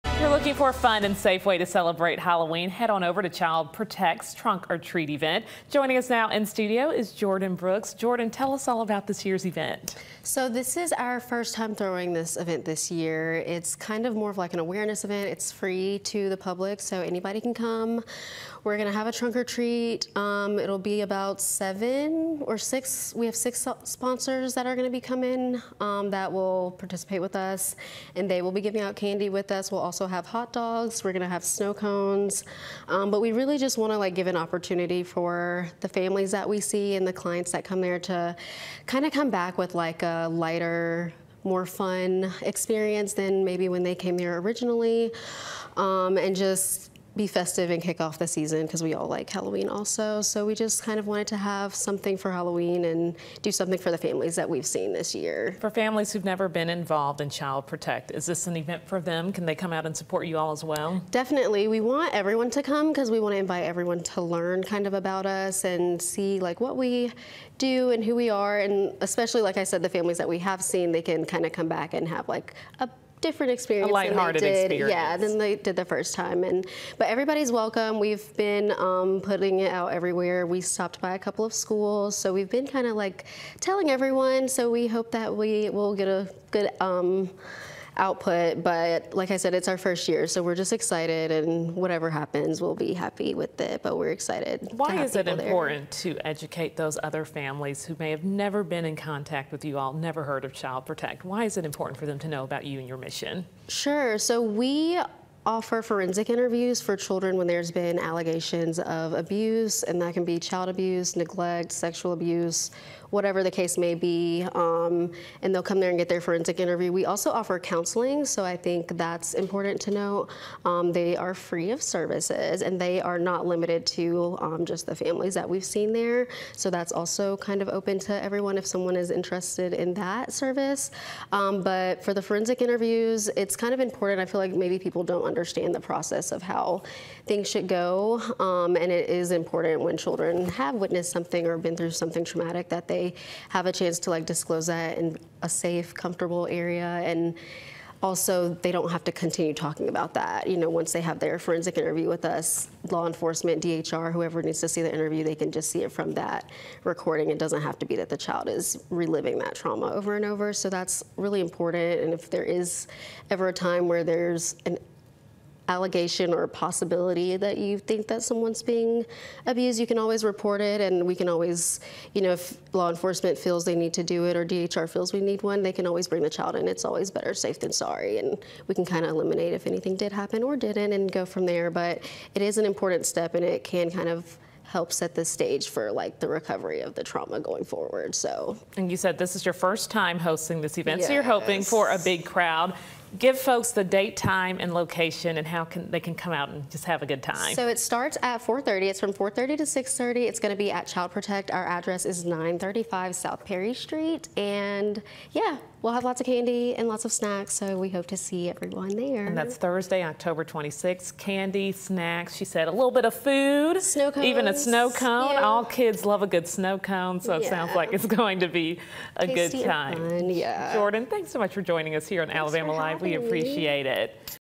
Child Protect trunk or treat event
Looking for a fun and safe way to celebrate Halloween? Child Protect is hosting a trunk or treat!